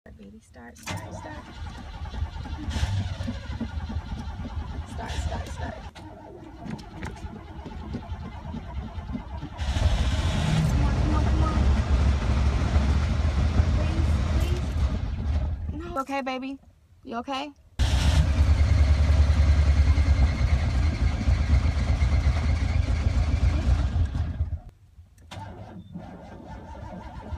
May 29, 2021
the Buick is still giving them fits as it will not start up. Chris is being told to put his seat belt on so that if step-mom ever gets the car to finally start he will be strapped in. When step-mom manages to get the car started, it stalls right back out on them.

This actually could be good for Chris because if step-mom is unable to get the car started it will mean that he won't have be seen exiting the car & risk it stalling on his step-mom in front of his pals